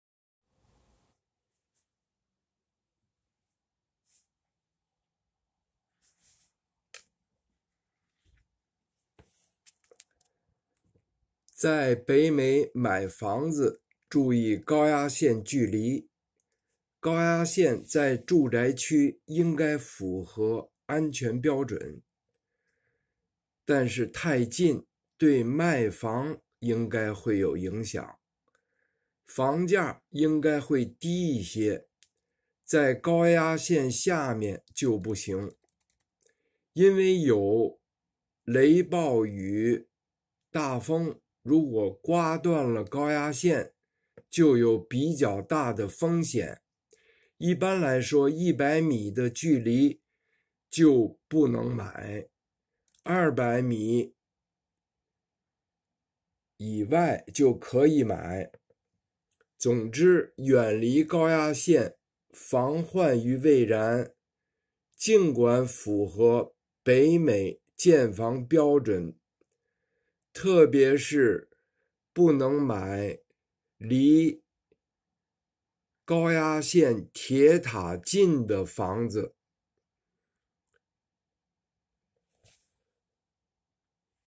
March 15, 2021 在北美买房子注意高压线距离 高压线在住宅区应该符合安全标准。但是太近对卖房应该会有影响，
在北美买房子注意高压线距离
高压线在住宅区应该符合安全标准。
但是太近对卖房应该会有影响，
房价应该会低一些。
在高压线下面就不行，
因为有雷暴雨大风如果刮断了高压线，
就有比较大的风险。
一般来说100米的距离就不能买。
200米以外就可以买。

总之远离高压线，防患于未然。
尽管符合北美建房标准。